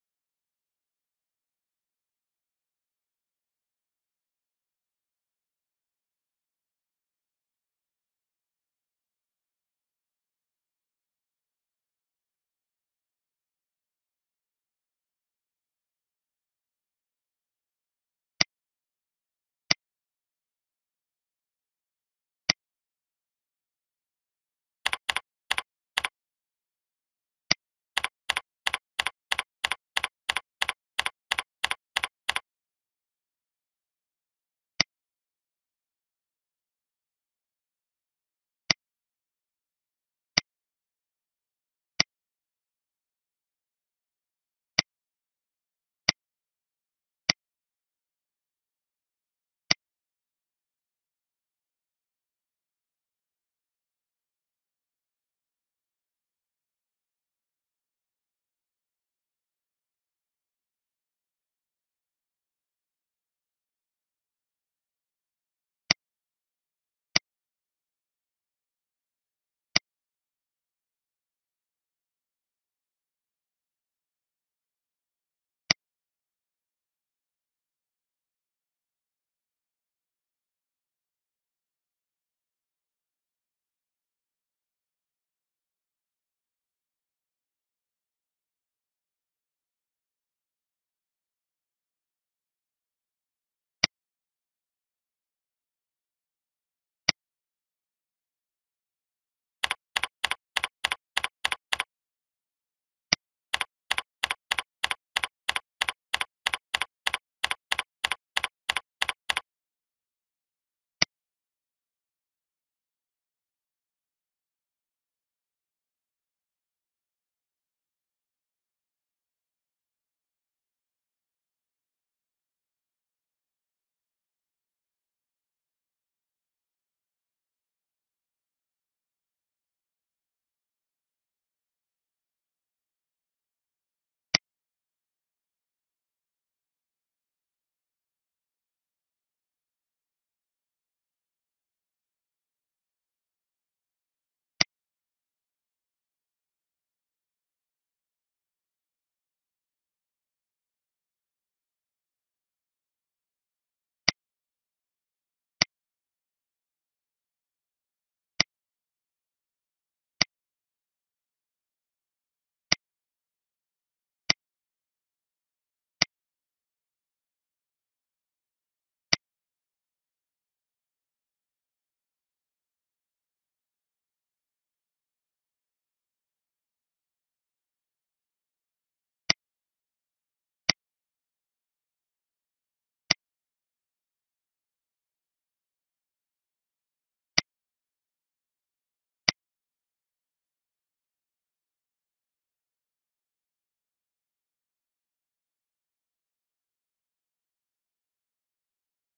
Coordonnées relatives dans les macros dans Excel - Formation Excel Marseille
Tutoriel vidéo sur la fonctionnalité d' Excel : Coordonnées relatives dans les macros, issu de la formation Excel de Optivalys Formations à Marseille.